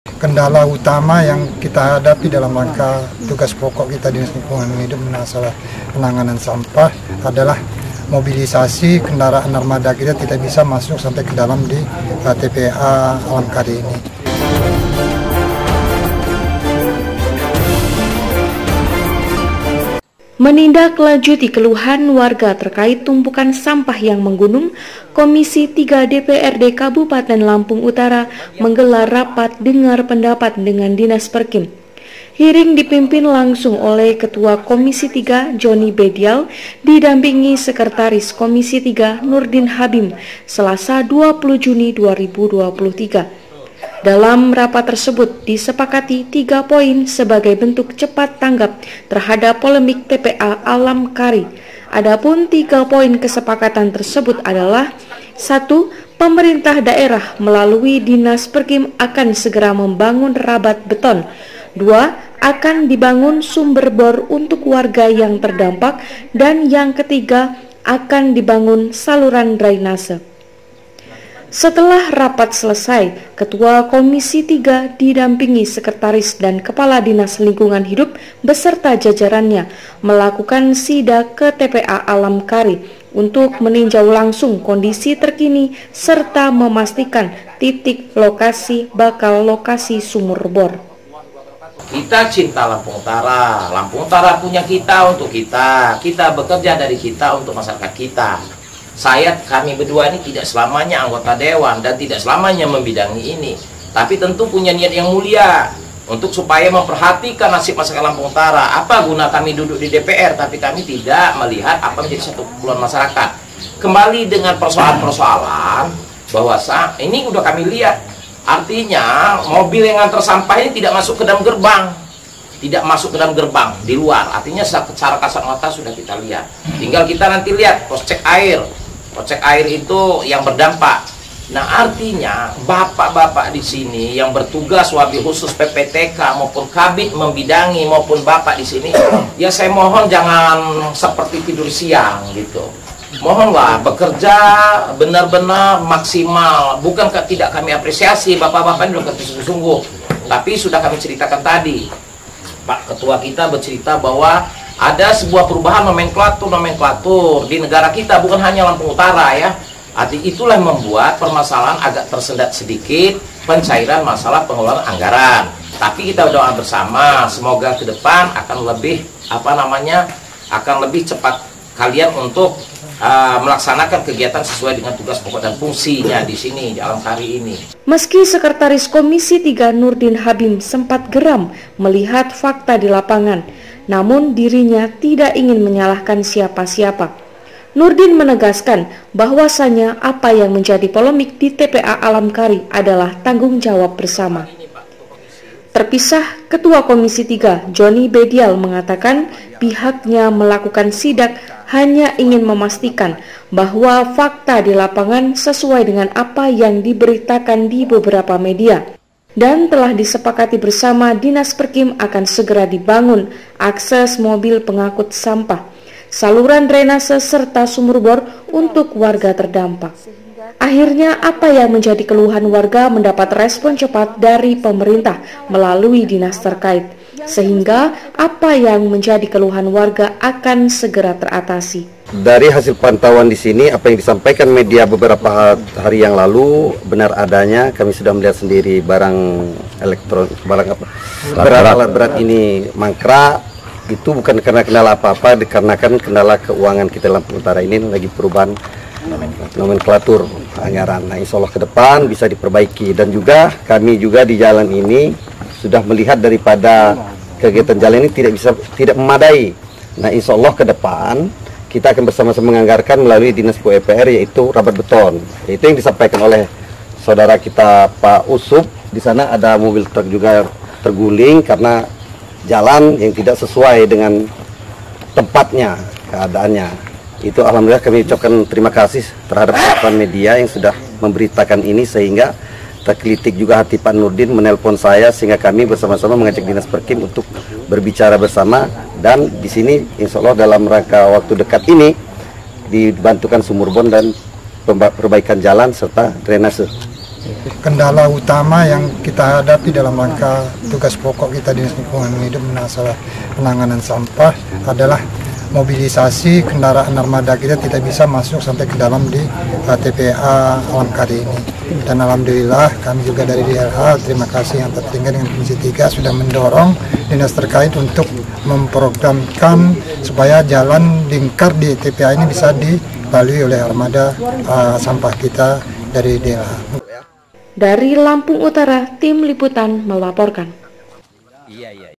AHIRNYA POLEMIK TPA ALAM KARI MENJADI SKALA PRIORITAS PEMBANGUNAN
INDEPENDEN / AKTUAL / INSPIRATIF